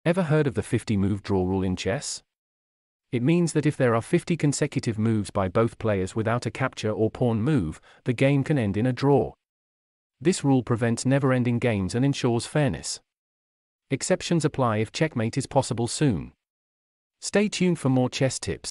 Unraveling the 50 Move Draw Rule in 20 Seconds!
In this captivating YouTube short, we dive into the intriguing world of chess and unravel the enigmatic 50 Move Draw Rule in just 20 seconds! Prepare to have your mind blown as we break down the intricate details of this rule that keeps grandmasters on their toes. Whether you're a chess enthusiast or a curious mind seeking knowledge, this concise and informative video will leave you with a clear understanding of this crucial aspect of the game. Get ready to elevate your chess IQ and explore the fascinating strategy behind the 50 Move Draw Rule!